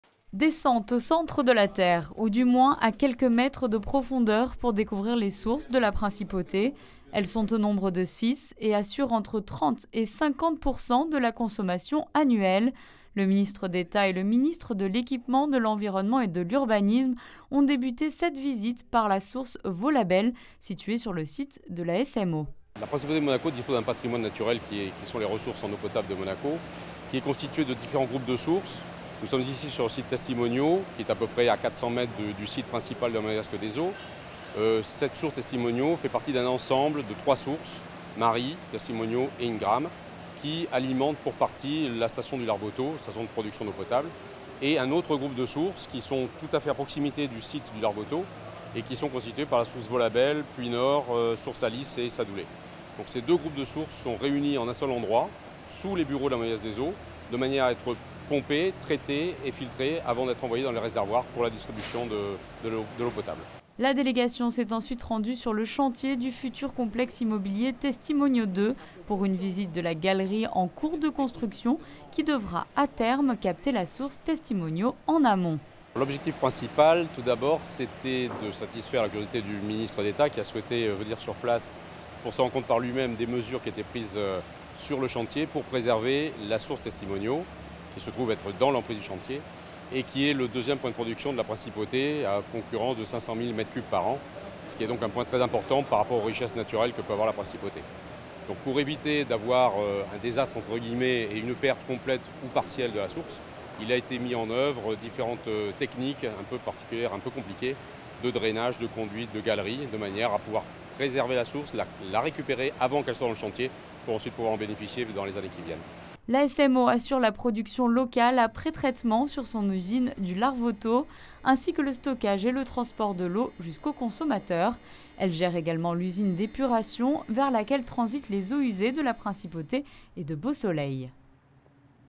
Descente dans les sources de Monaco
Le Ministre d’État, S.E. M. Serge Telle, et le Conseiller de Gouvernement - Ministre de l’Équipement, de l’Environnement et de l’Urbanisme Marie-Pierre Gramaglia, sont descendus à quelques mètres de profondeur pour découvrir les sources de la Principauté et notamment la source Vaulabelle. Ces sources, au nombre de six, assurent entre 30 et 50% de la consommation annuelle d’eau potable à Monaco.